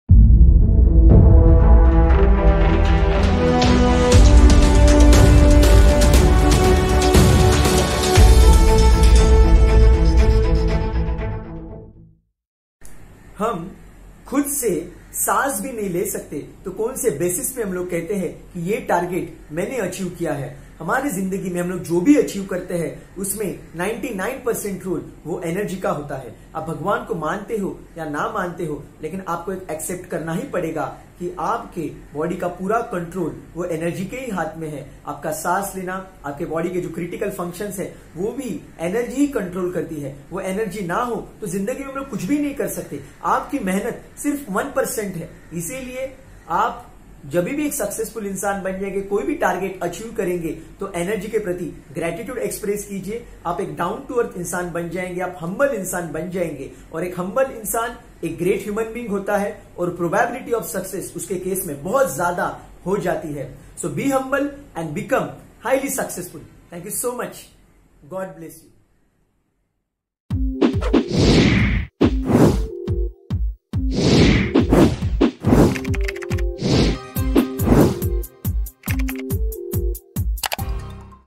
Why to be humble always ???(Hindi language)
Achieve whatever you want in life but never ever forget to remain humble in life...